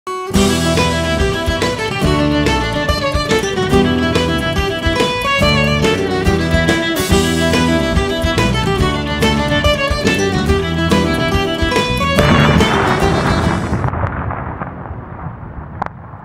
Патрон продовжує розміновувати території поблизу Чернігова
Минулої доби він разом з чернігівськими піротехніками ДСНС знищив 262 вибухонебезпечних предмети.

Дякуємо, друже, за службу🐾!